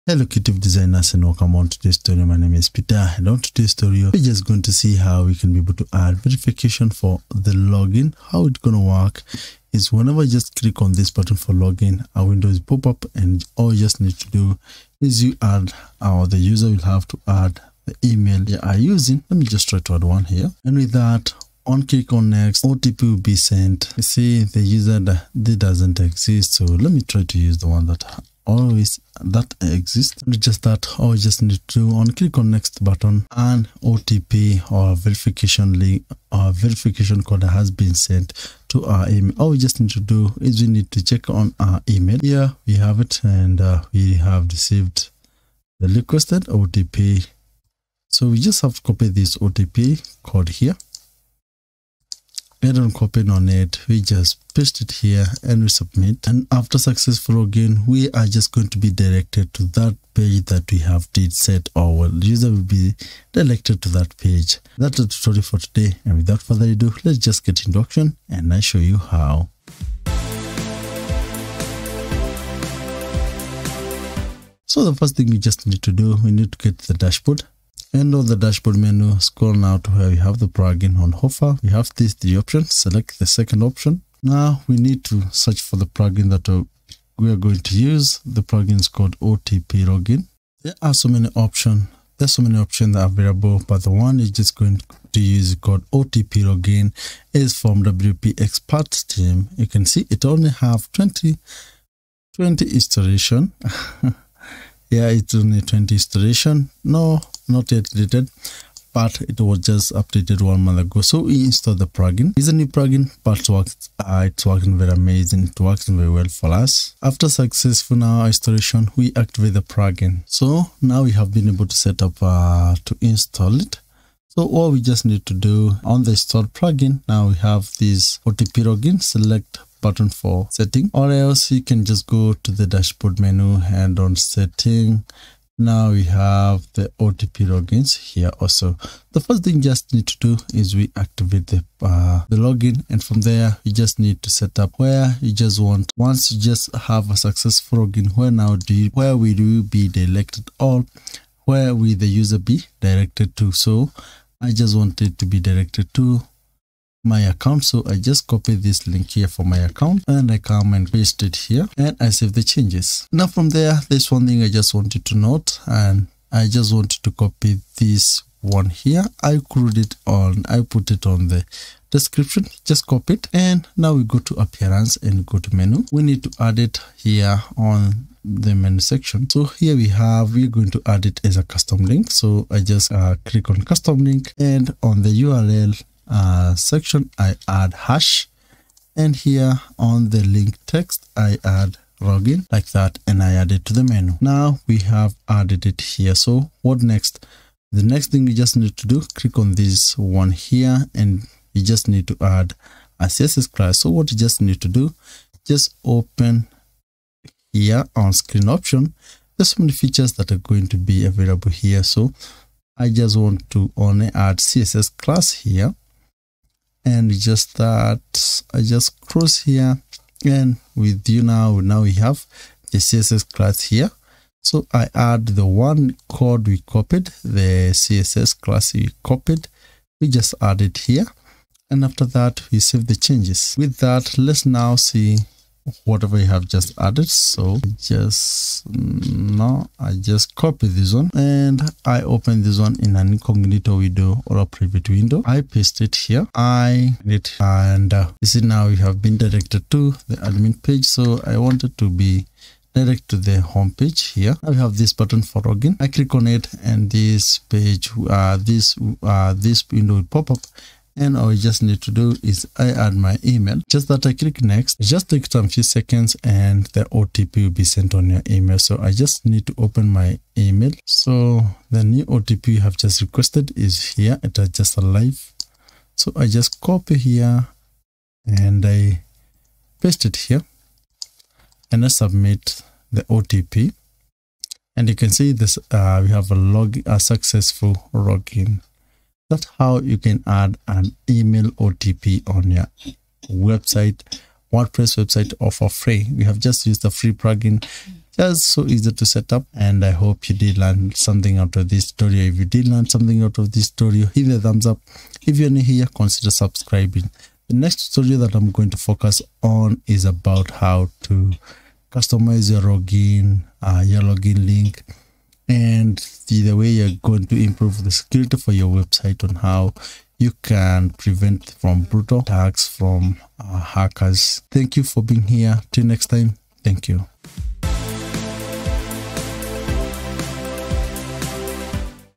Allow WordPress users to Login with FREE Email OTP verification.
This will work for all WordPress website and e-commerce websites. We are going to use free Plugin for use to achieve that. WordPress user verification will help on user verification during user login to the dashboard.

►► Shared with ❤️ by Thiarara ◄ ◄
✅How to add Custom Order Status to your WordPress WooCommerce eCommerce